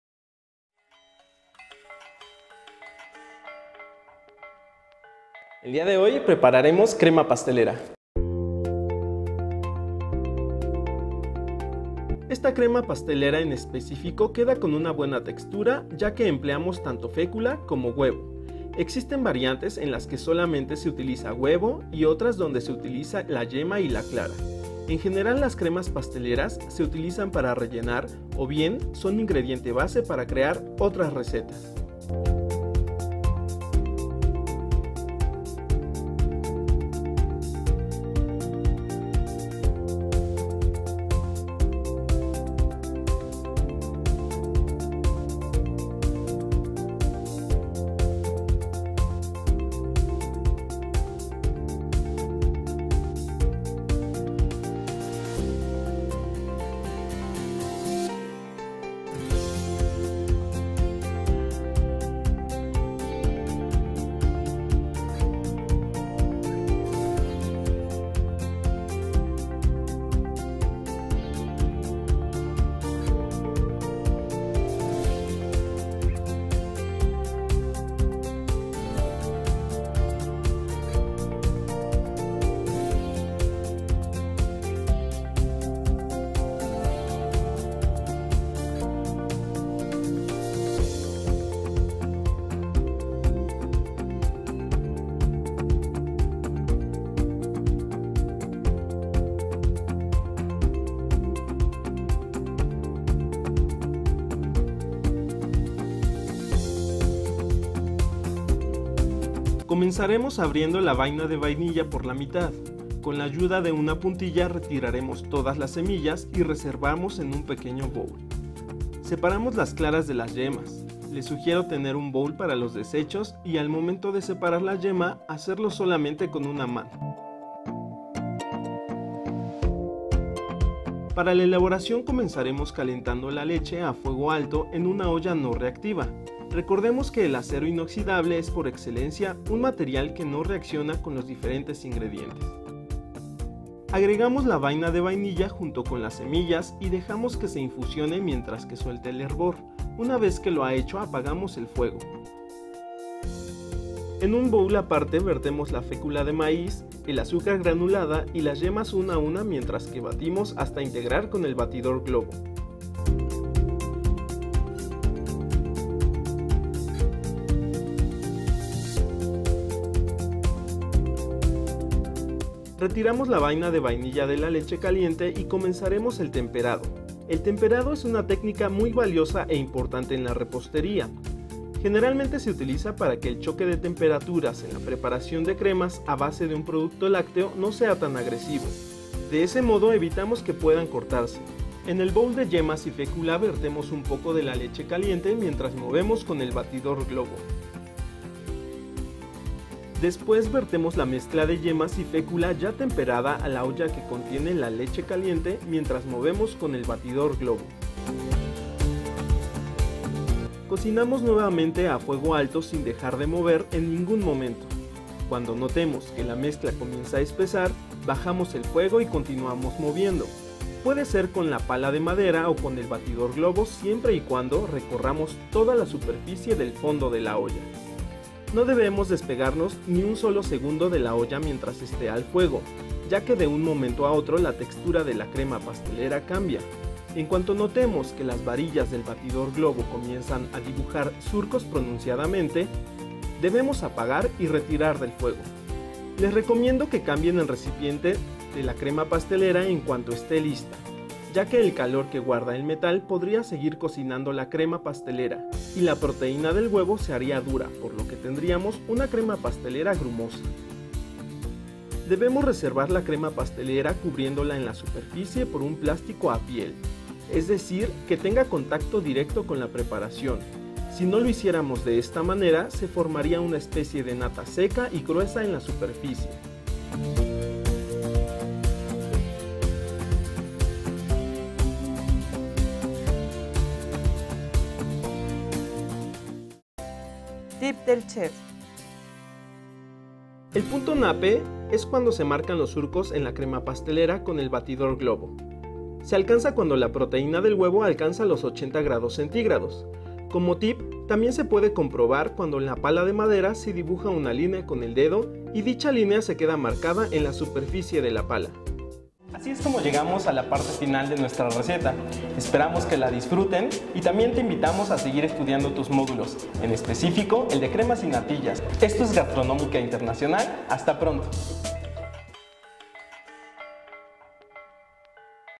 Crema pastelera
En el video se muestra la manera de elaborar una de las preparaciones más clásicas de la repostería: la crema pastelera que sirve como relleno para tartas, panes y otros postres.